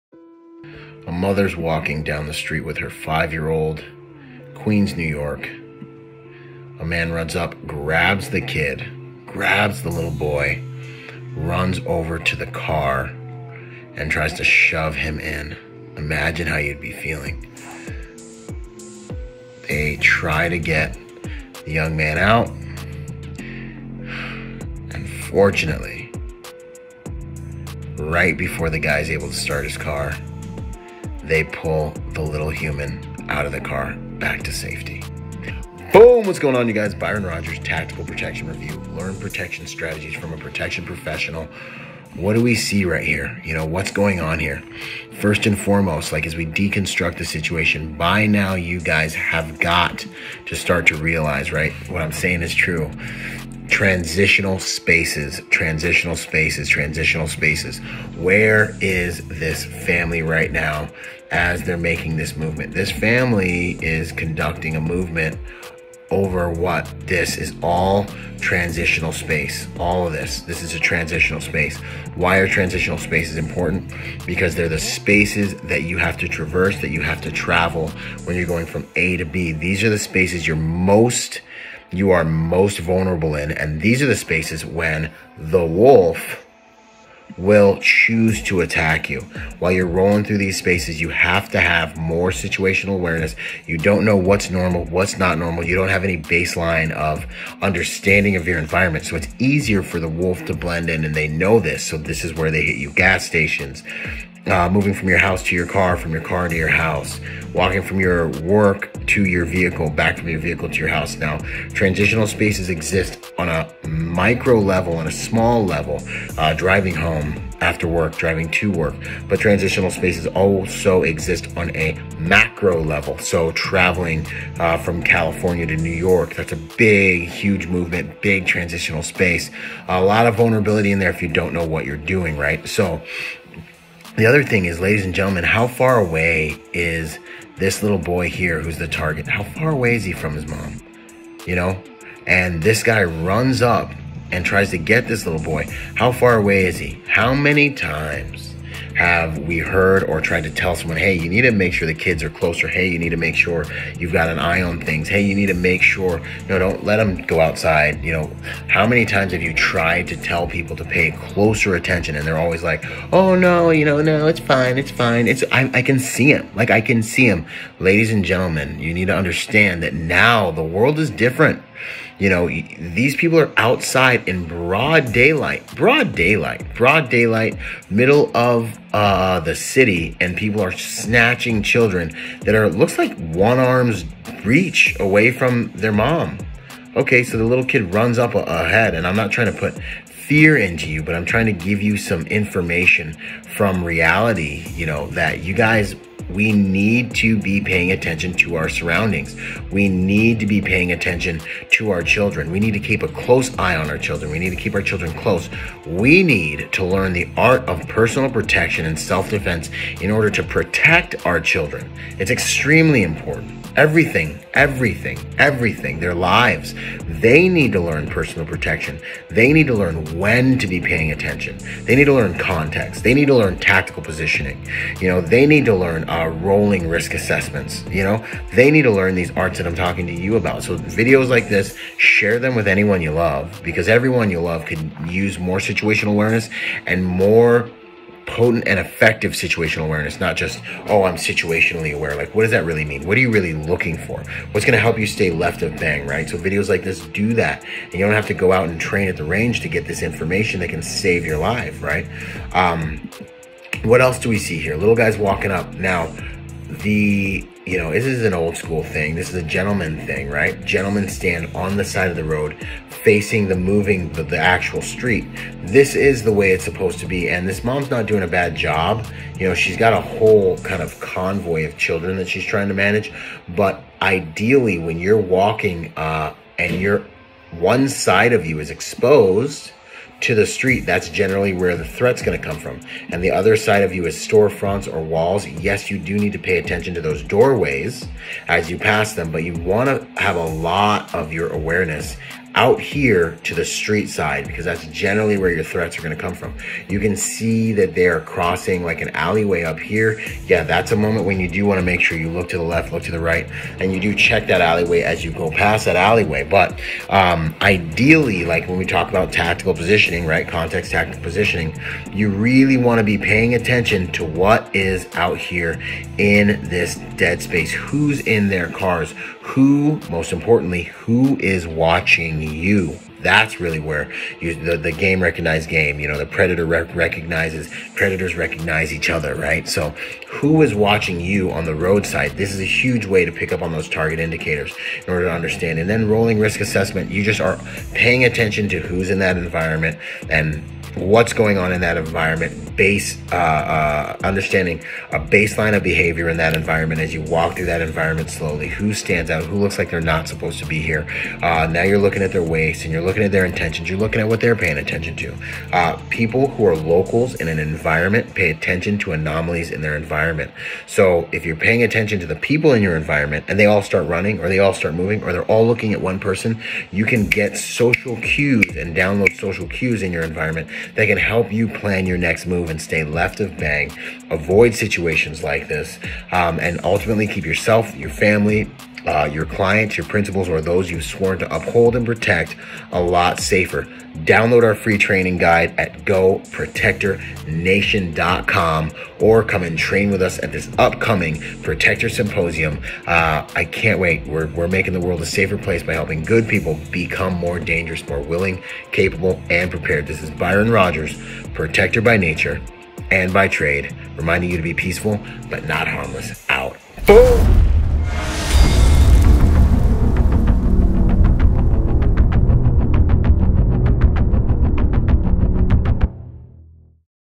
Kid Snatchers - Tactical Protection Review 🔴
⚜️ If you’ve been following the Tactical Protection review series for any time now you’re starting to recognize that there is a theme to violent encounters. Most of the time they happen in transitional spaces. Tactical positioning always plays a part. The context of the situation is usually the first hint as to whether or not a person should be ready. If they are not running a rolling risk assessment human beings are much riper to be victimized.

Many people invest lots of time and money in the hard skills of protection but the reality simply is that if you learn the soft skills you can avoid a huge percentage of danger both on and off the job.

PROTECTOR by nature and by trade
Byron Rodgers

⚜️PS4 iNSTRUCTORS include:
⭕️ Lt. Col. David Grossman

IG Go follow @executive_protection_lifestyle @bravo_research_group @byron_rodgers